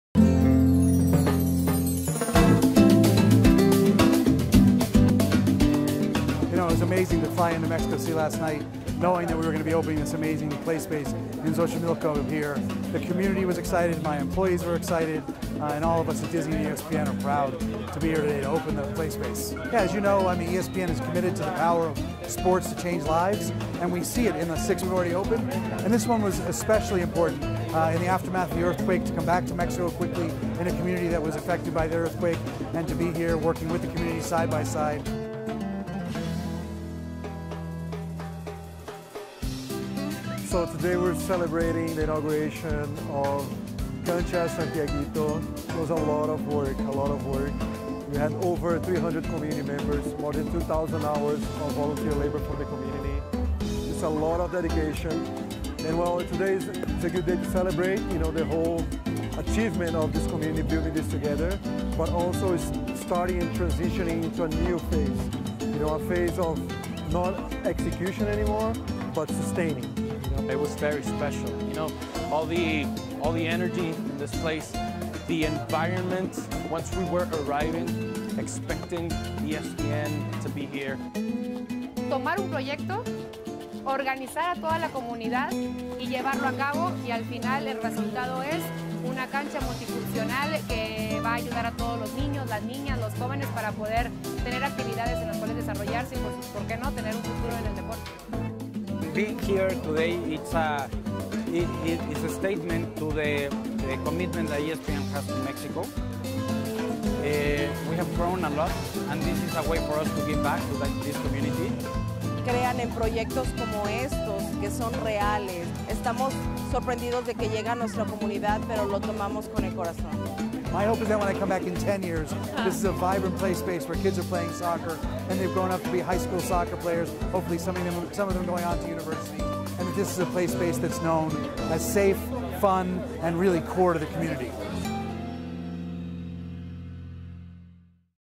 [love.fútbol] ESPN INAUGURATES NEW SPORTS SPACE IN CIUDAD MÉXICO, XOCHIMILCO
August 7, 2018: Community leaders, local residents, and youth gathered in Ciudad México, Xochimilco to participate in the opening of a new multifunctional sports court dedicated to the local community and its residents. The global project, led by ESPN, has seen six spaces built across Latin America and one in India.

Video credit: Cactus Film
Chat with us!